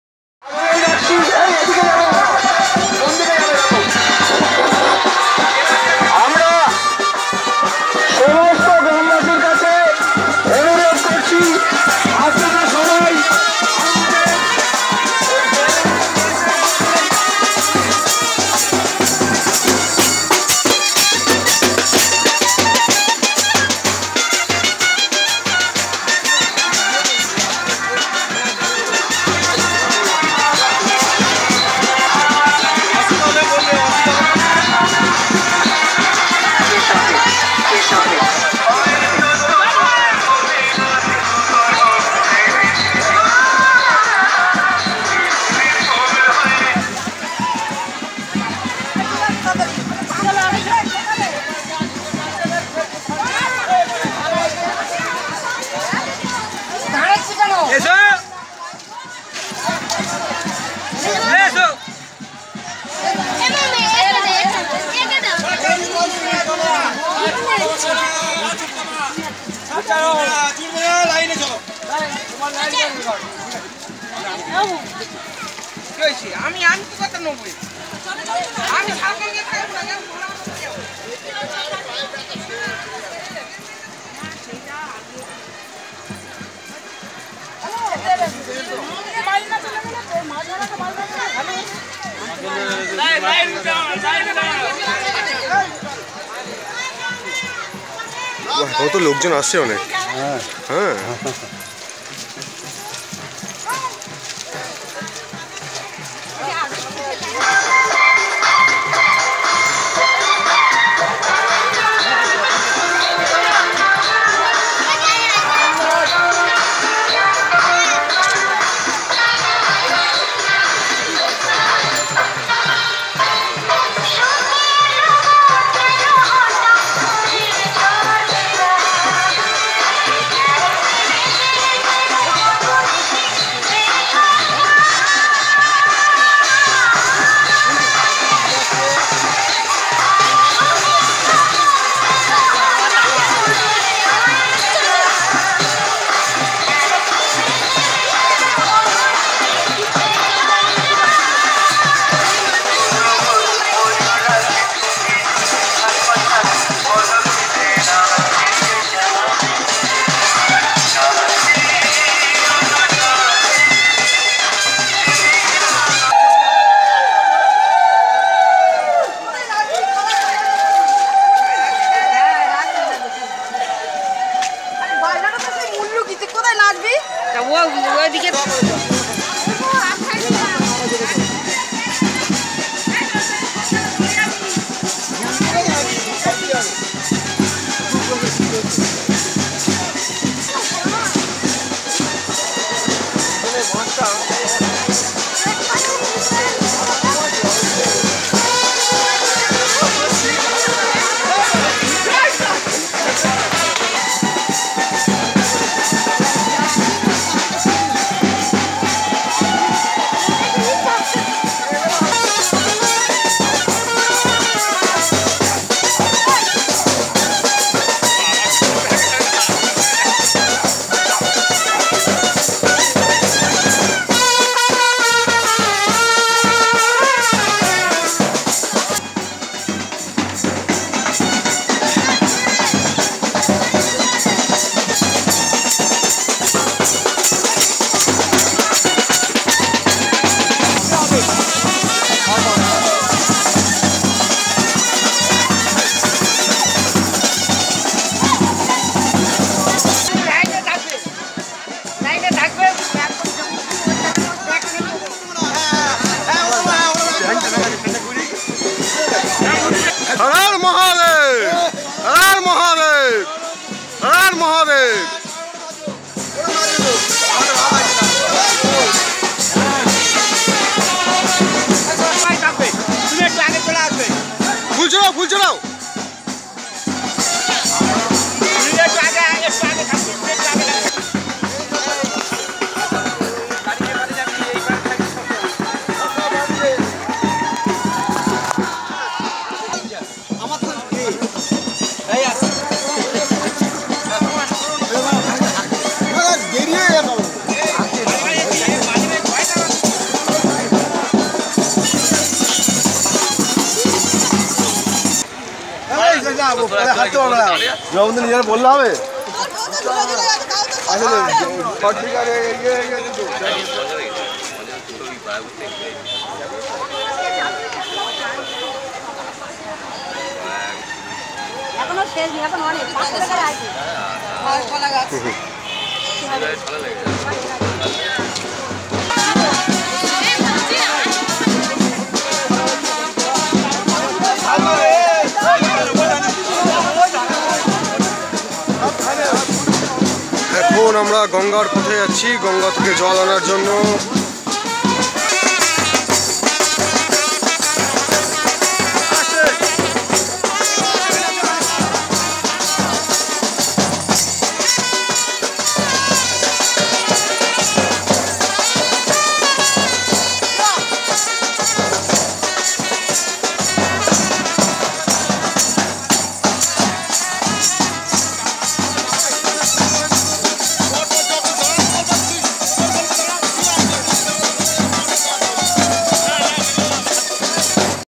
CORONA VIRUS FAILED !!! In The Name Of Lord Shiva Human Gathering
near about thousands of people gathered together in the name of lord shiva to fight against corona virus.